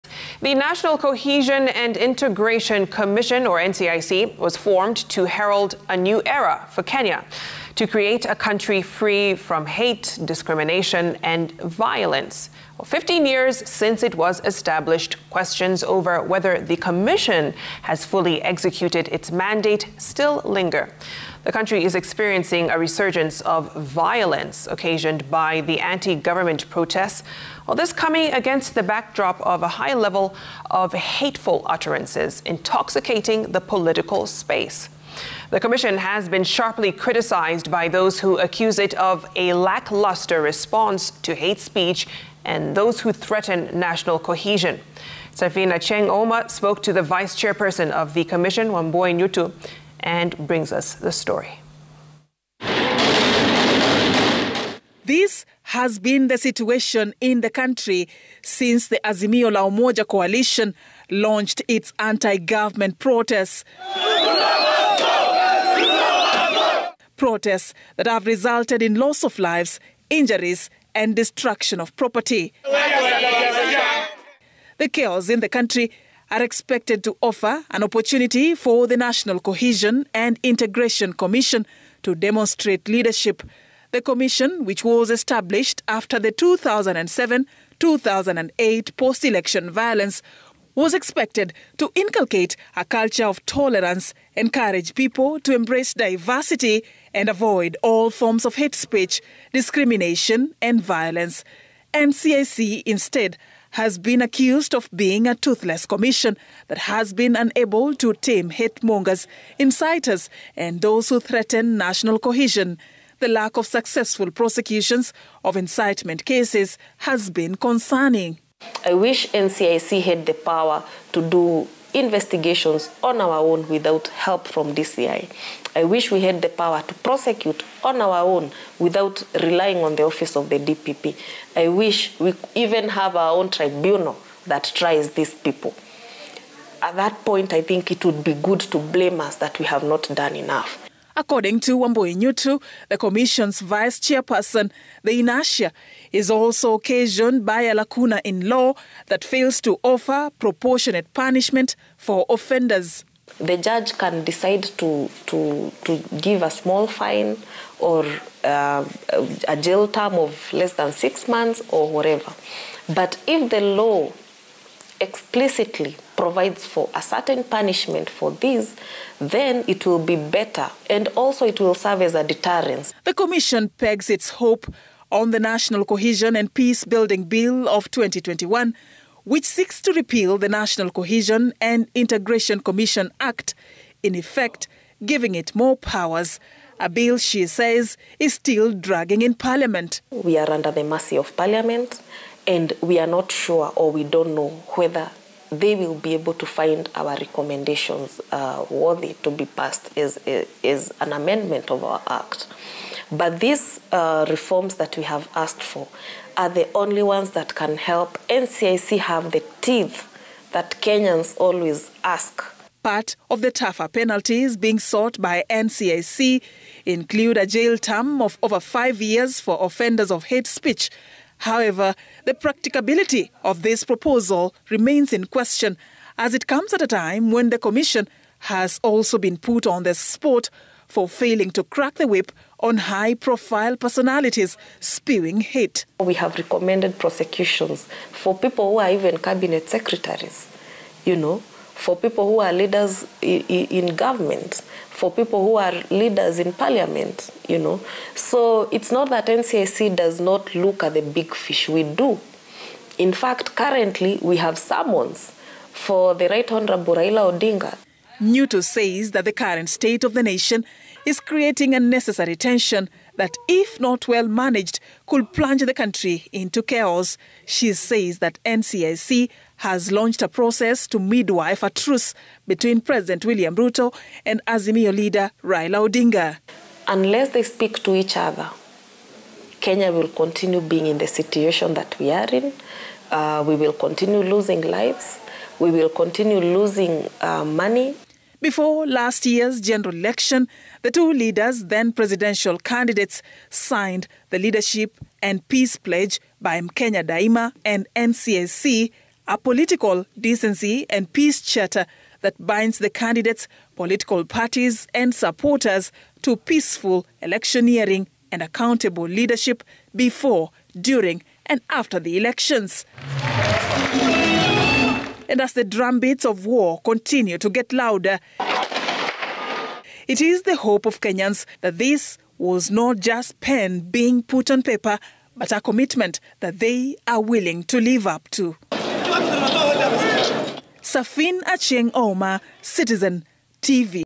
NCIC was established to promote national unity
After the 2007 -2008 post-election violence, the National Cohesion and Integration Commission was formed to herald a new era for Kenya, to create a country free from hate, discrimination and violence. 15 years since it was established questions over whether the commission has fully executed its mandate still linger.